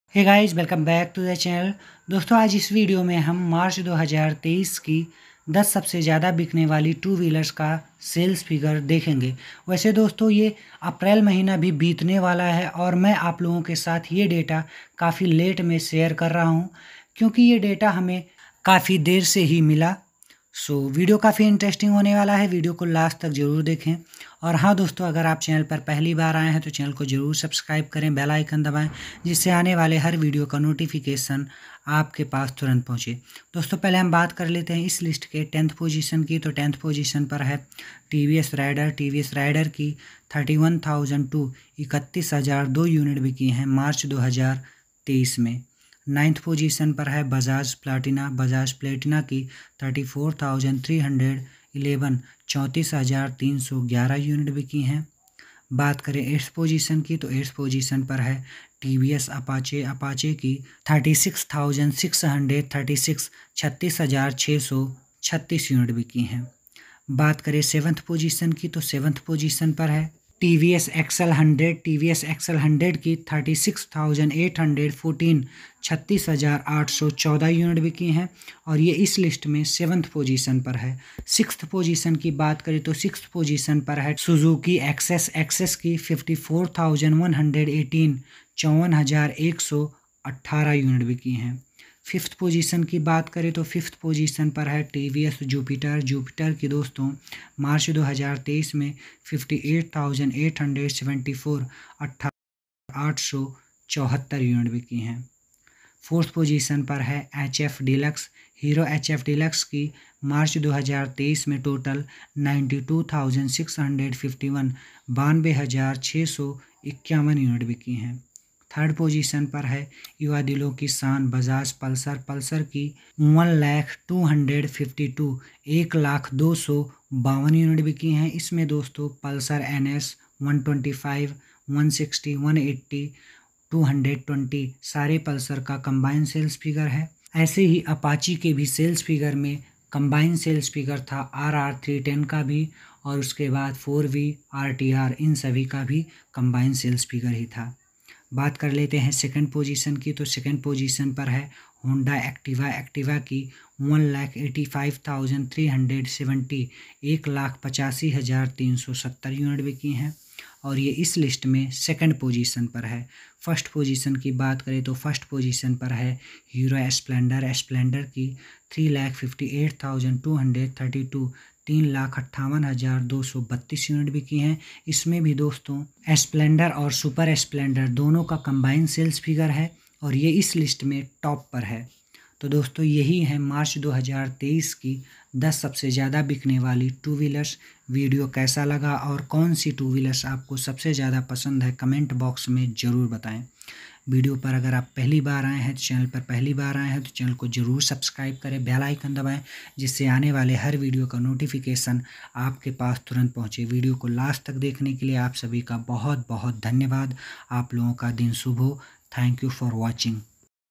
Top 10 selling two wheelers march 2023.
Here is top 10 selling bike sales list of march 2023